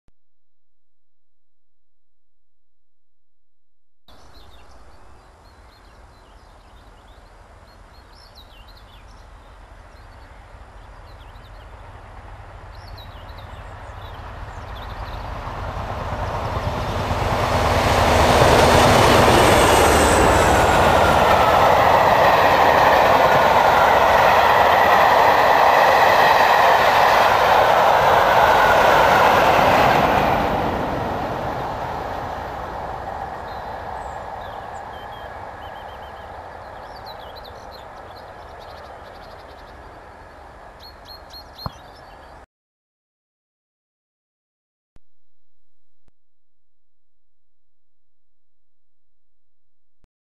47798 East Markham 1Z46
47798 1Z46 East Markham on a Doncaster - Canterbury railtour. 23/5/09